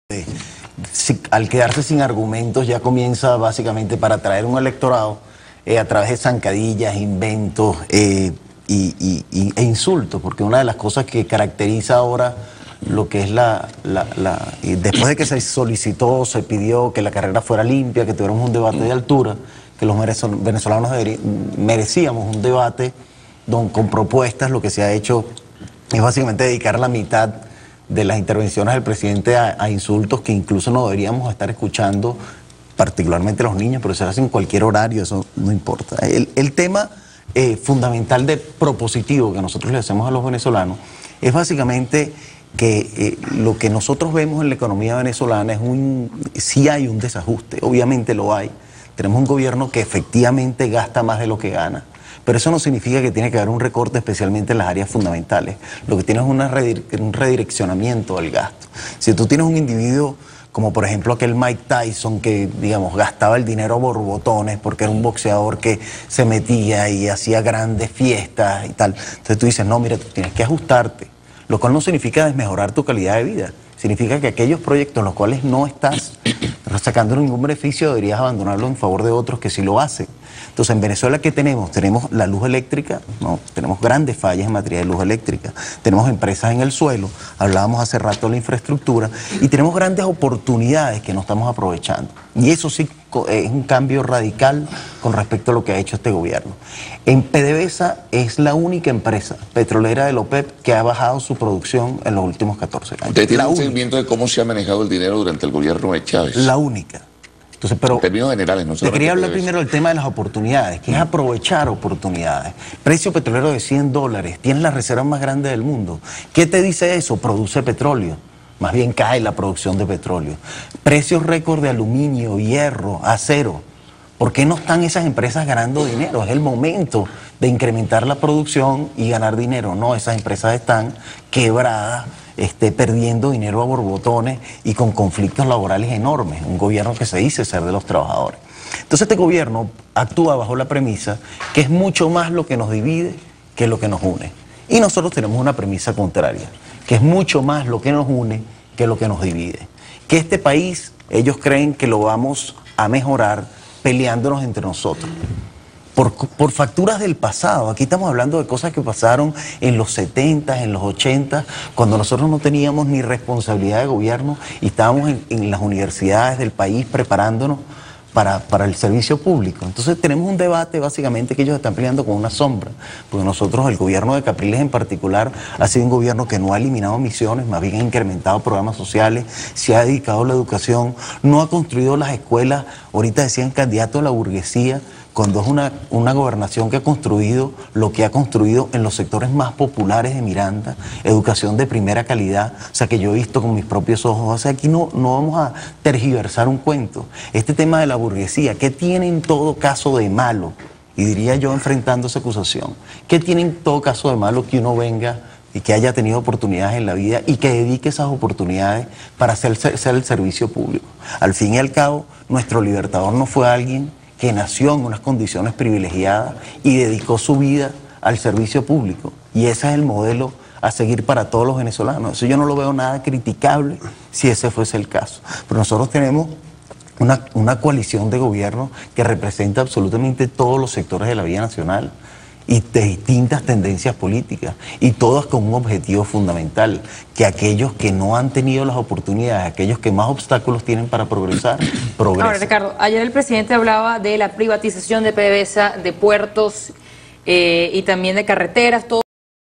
RicardoVillasmil_x264.mp4
El Coordinador del Programa de Gobierno, Ricardo Villasmil, hablo sobre los planes económicos que implementará el candidato presidencial de la oposición en su eventual periodo de gobierno y desestimó el supuesto documento que calificó de forjado que maneja los oficialistas sobre el proyecto del Comando Venezuela